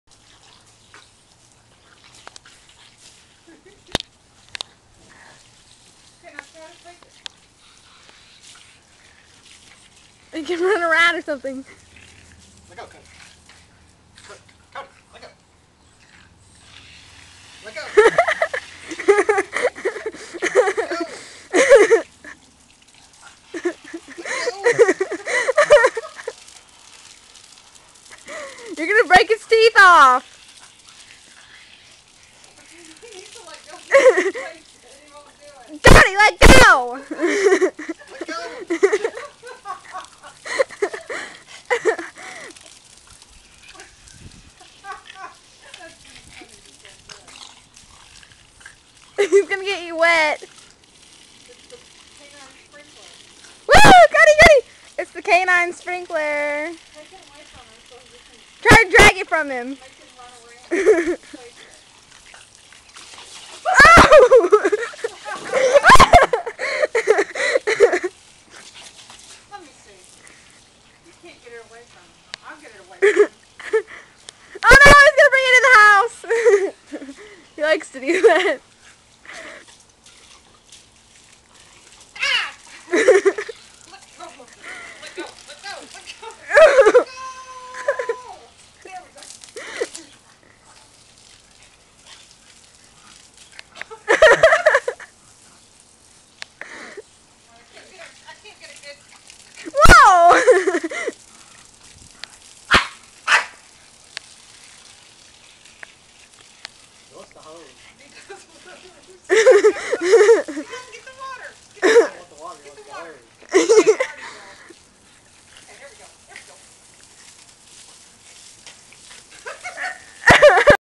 Cody "The Hulk" vs. the Hose
Cody, the Boston Terrier goes crazy when the hose is on!  He will go on like this for 45 min. or until he has worked himself to exhaustion.